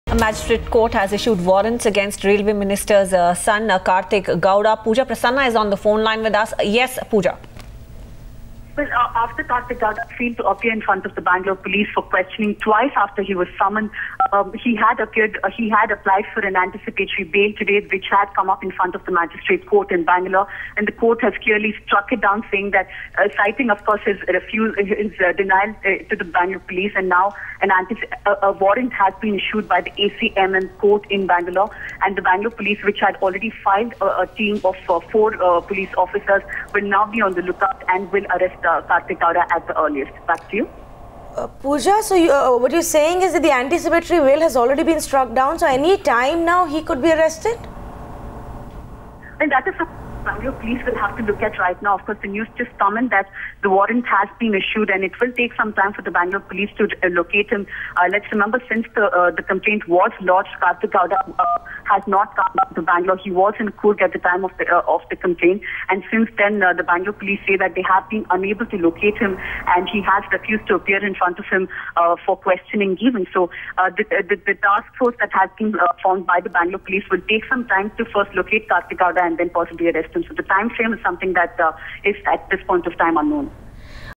Warrant against Karthik Gowda
Karthik Gowda, son of Railway Minister DV Sadananda Gowda, accused of rape and cheating a Kannada actor, failed to appear before the police even as the second notice to him expired on Wednesday (Sept 3). The police now are expected to file objection to the anticipatory bail application filed by Mr. Karthik Gowda that is coming up for hearing on Thursday (Sept 4). Social Media Links :-